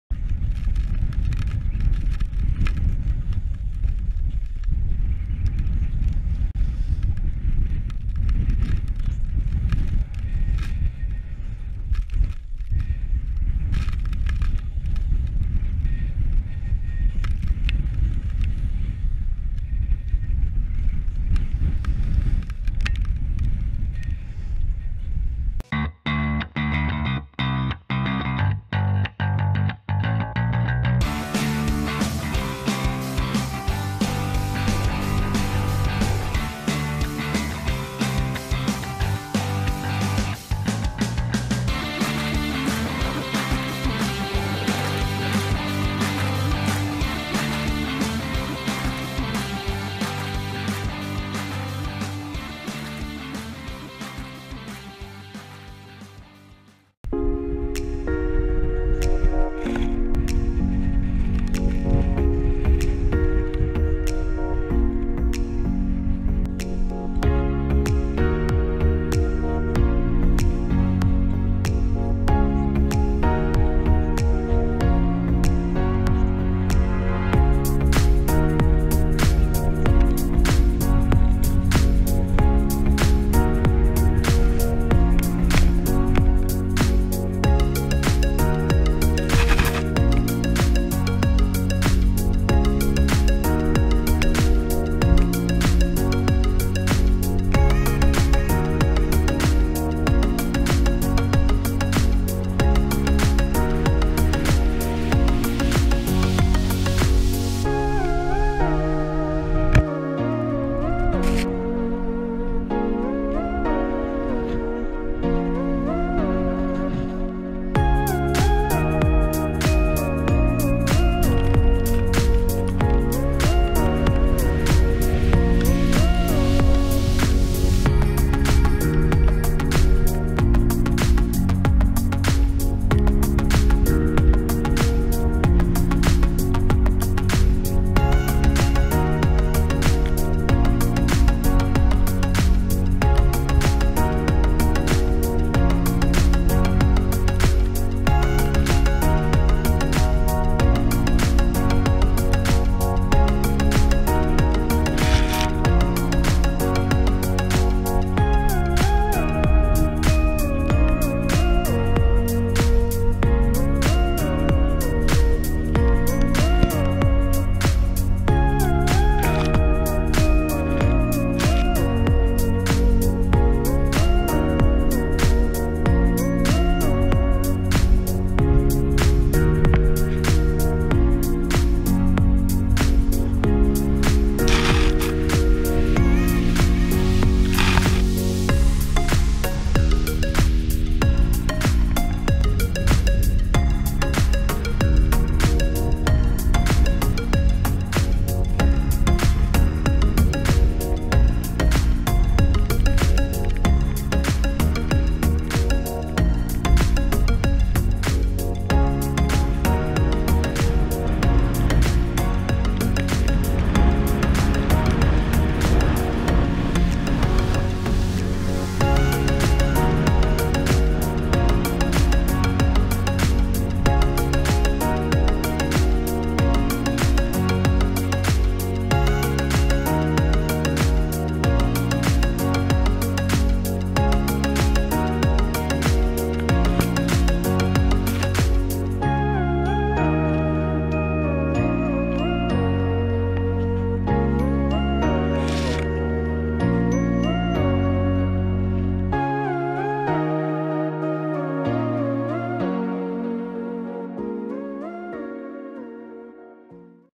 Killer Conditions in Steamboat Springs This Year! Snowboarding
I am lucky enough to call Steamboat Springs, CO my home, and this year has been great! I got to ride just about 20 days, and more than half of those were pretty great powder days.

These are just a few clips from one of those days...

Board: Salomon Grip